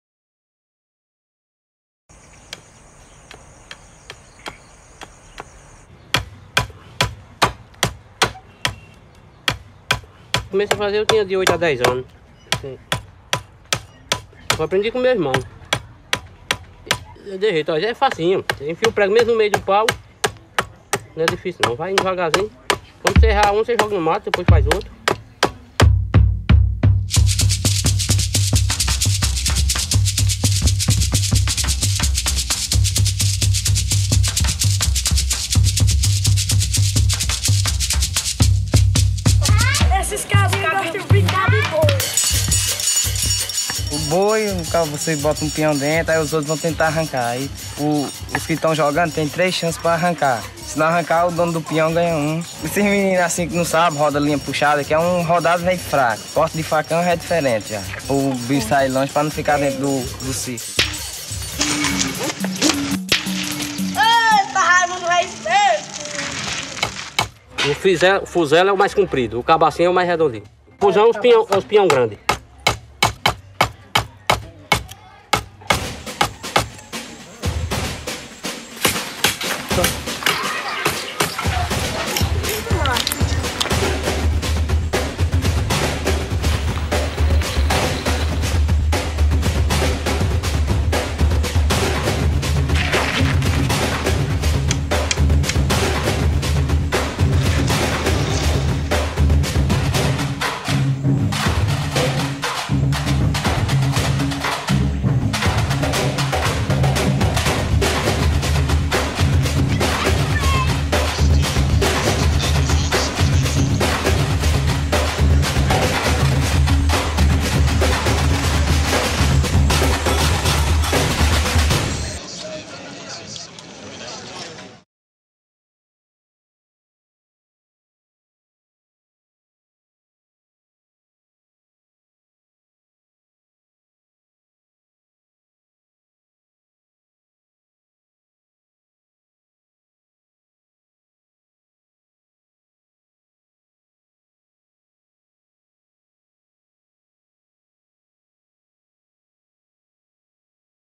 Terra de cabinha - BATALHA DE PIÃO
Como se faz e se joga pião: ensinado por crianças e mestres do Cariri - Conteúdo complementar ao livro Terra de cabinha: pequeno inventário da vida de meninos e meninas do sertão.